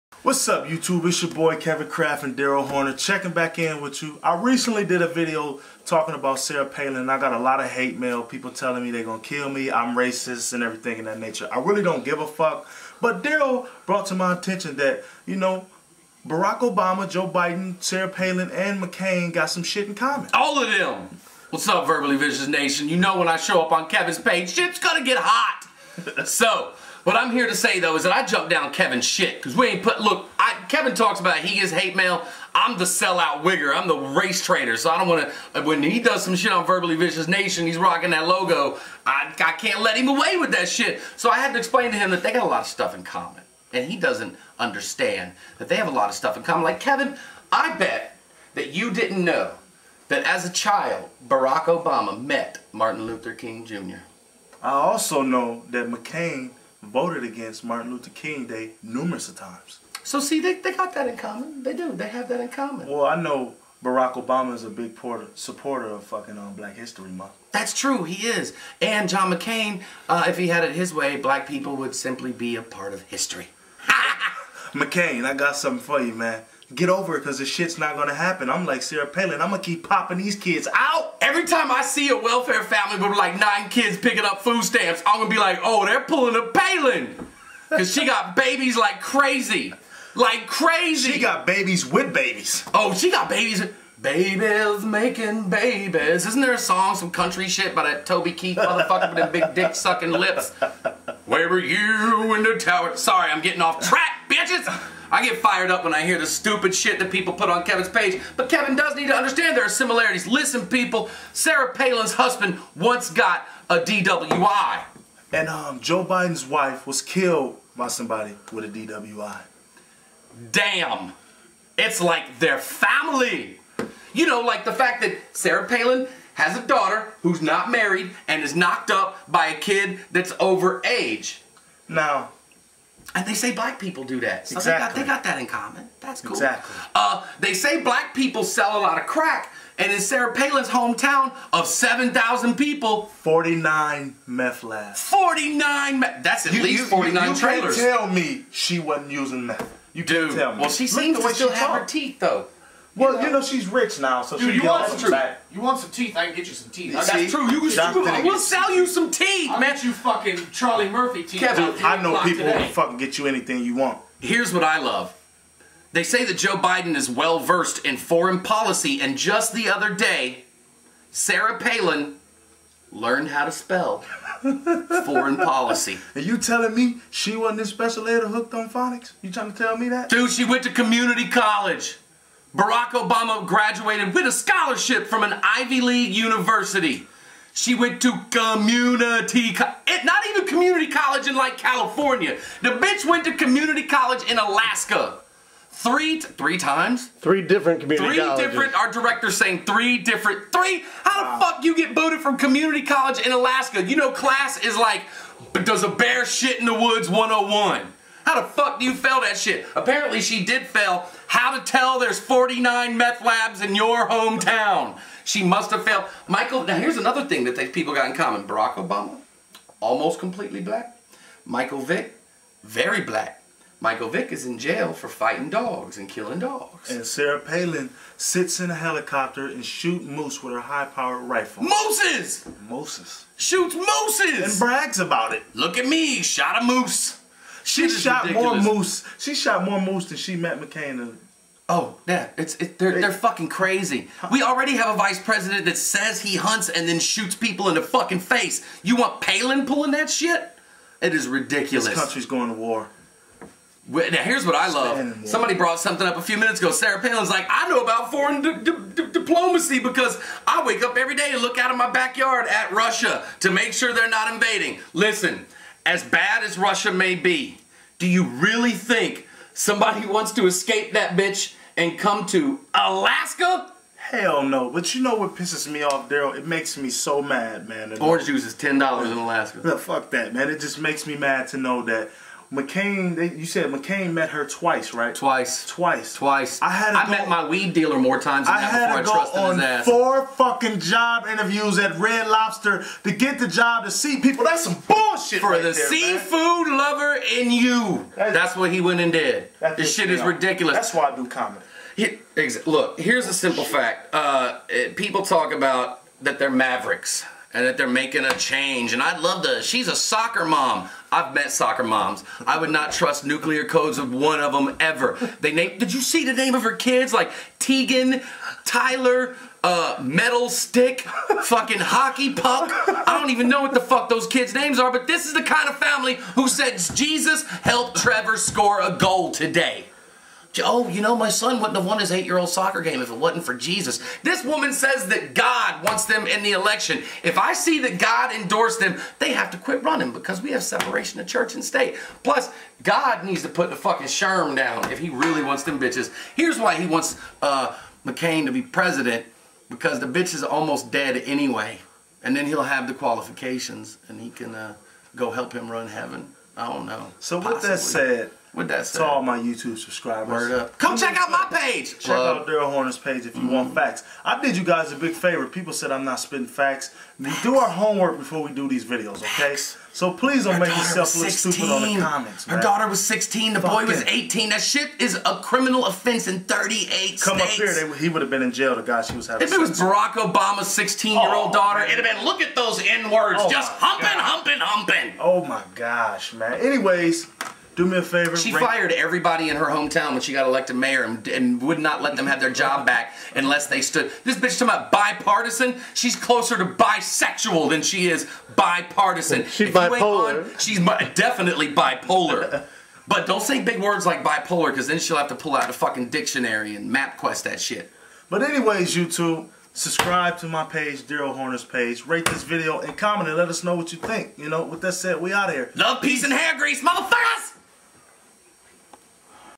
Re: Sarah Palin choked!.....(Exclusive Interview with Katie Couric!)
Daryl and Kevin discuss the election